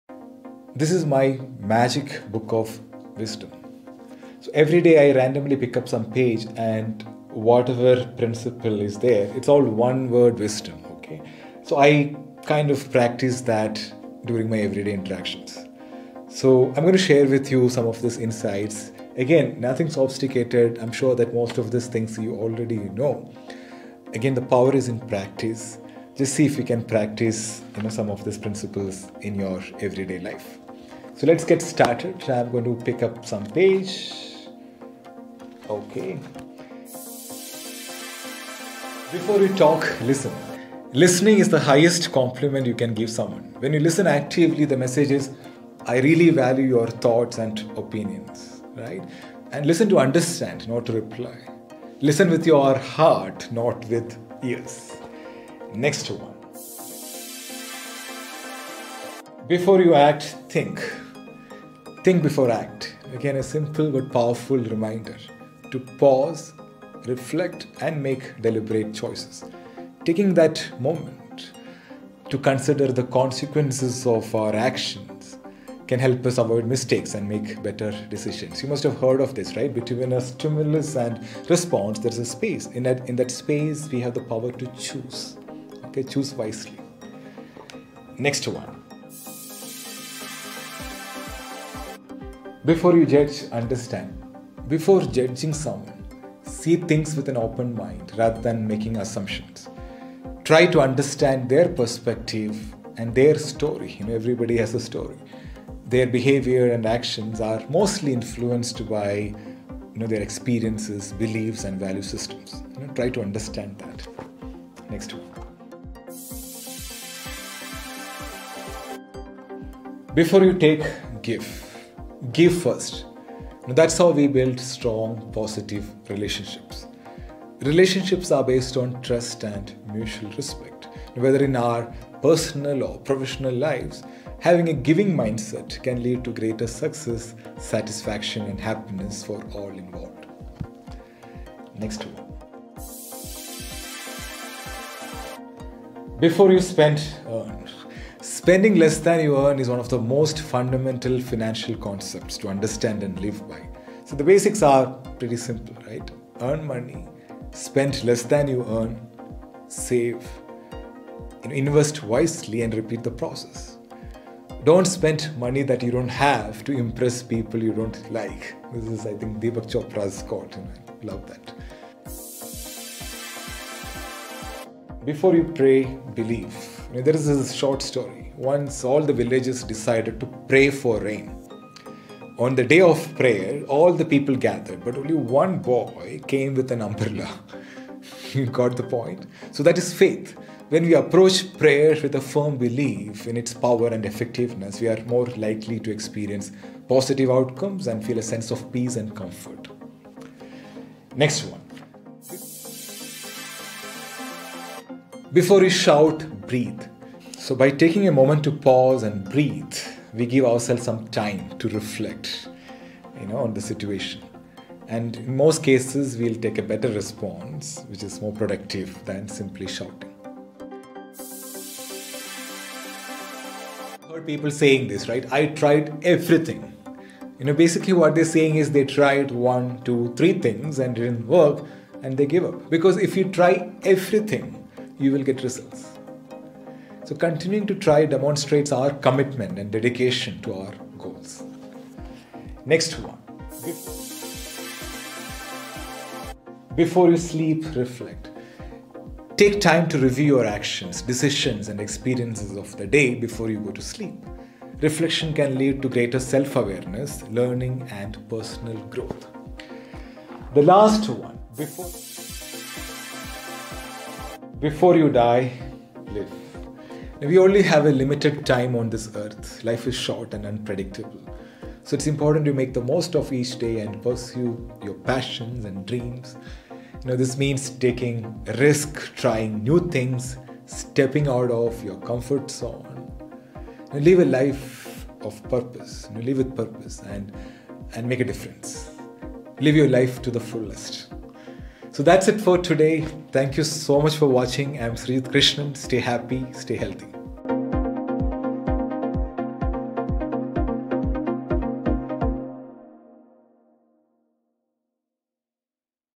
One-Word Wisdom I Sreejith Krishnan
One Word Life Principles from my little book of wisdom

1. Before You Talk - Listen
2. Before You Act - Think
3. Before You Judge - Understand
4.Before You Take - Give
5.Before You Spend - Earn
6.Before You Pray - Believe
7.Before You Shout - breathe
8.Before You Quit - Try
9.Before You Sleep - Reflect
10. Before You Die - Live